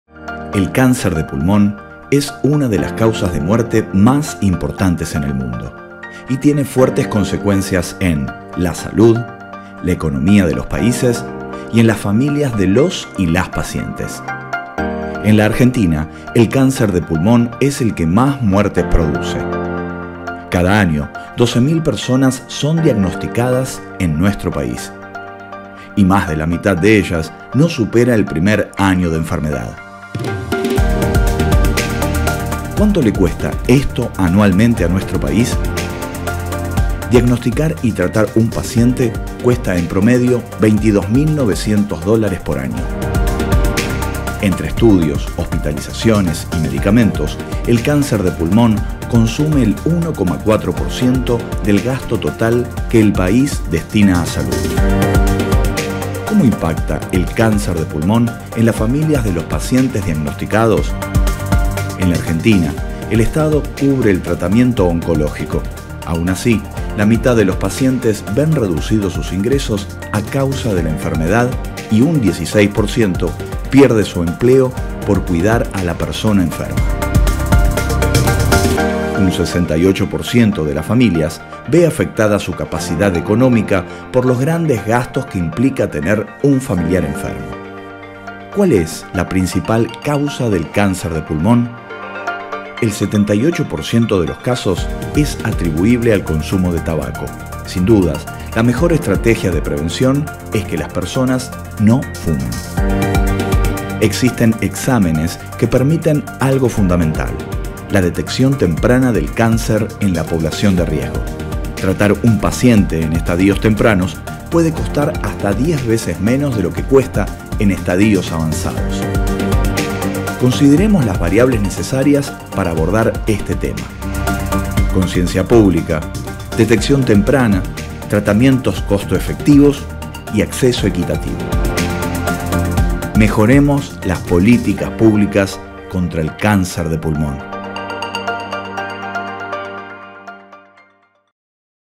¿Qué pasa con el cáncer de pulmón en la Argentina?
Es el tipo de cáncer que más muertes causa en el país y el 78% de los casos son atribuibles al consumo de tabaco. Cuánto cuesta diagnosticar y tratar un paciente con esta enfermedad, cuál es la importancia de la detección en etapas tempranas y cuál es el impacto en las familias.